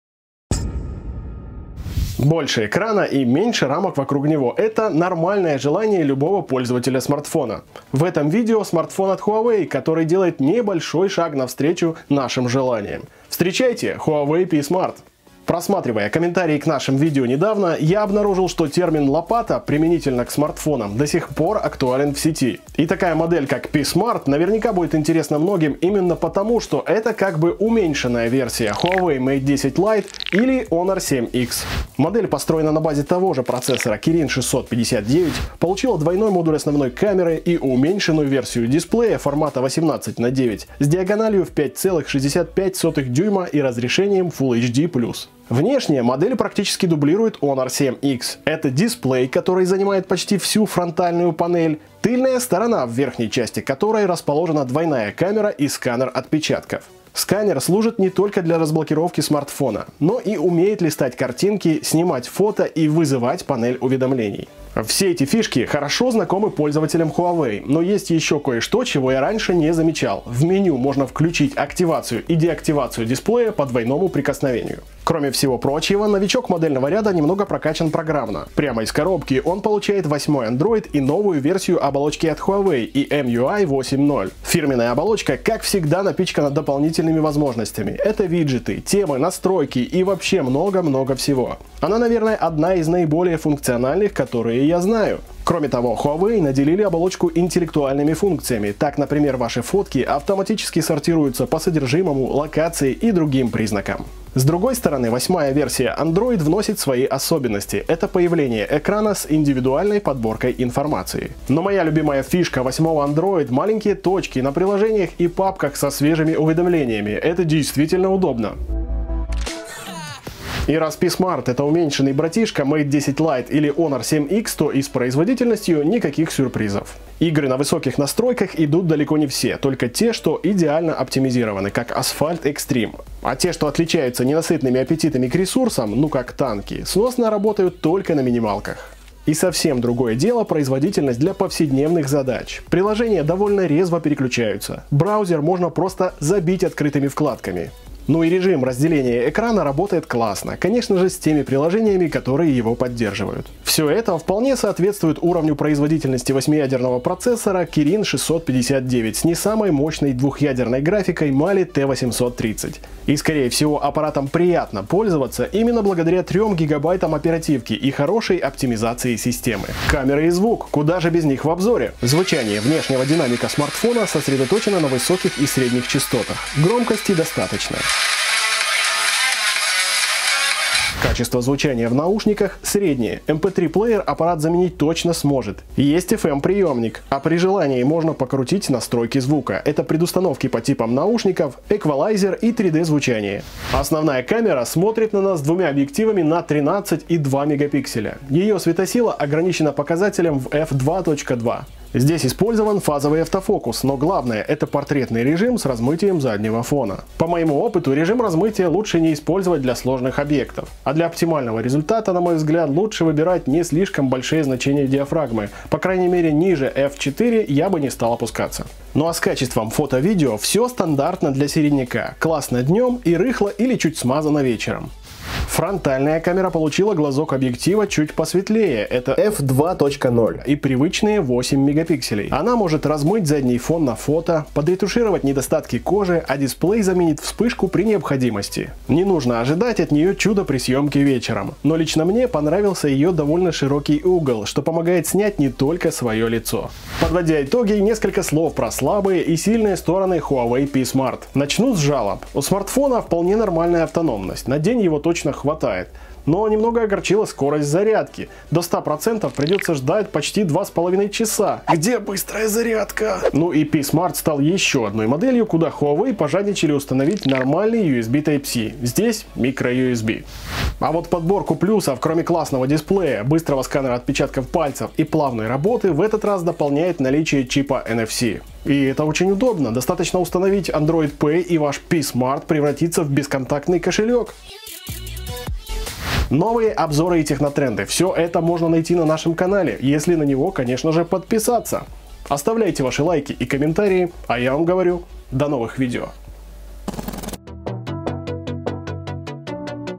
Обзор смартфона Huawei P Smart - компактный экран 18:9, двойная камера и NFC
Больше экрана, меньше рамок. Это вполне нормальное желание любого пользователя смартфона. И в этом видео смартфон от Huawei, который делает небольшой шаг навстречу нашим желаниям. Встречайте - Huawei P Smart. Больше подробностей в новом обзоре от ttt.ua